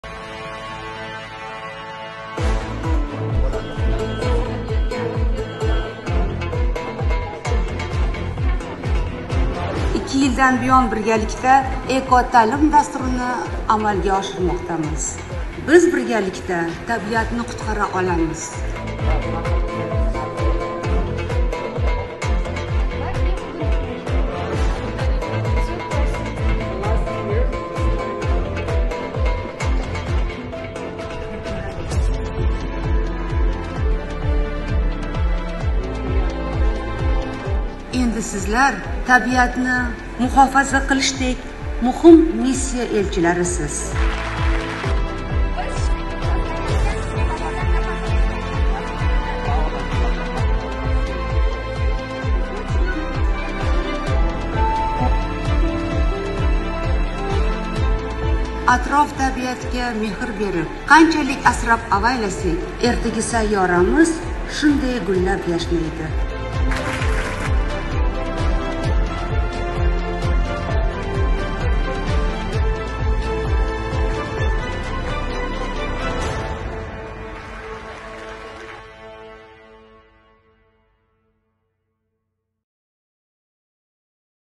Eko-IT oromgohi ishtirokchi
Ziroat Mirziyoyeva Eko-IT oromgohi ishtirokchilariga: "Sizlar tabiatni muhofaza qilishdek muhim missiya elchilarisiz"
Зироат Мирзиёева - участникам Эко-IT-лагеря: "Вы - послы такой важной миссии, как защита природы"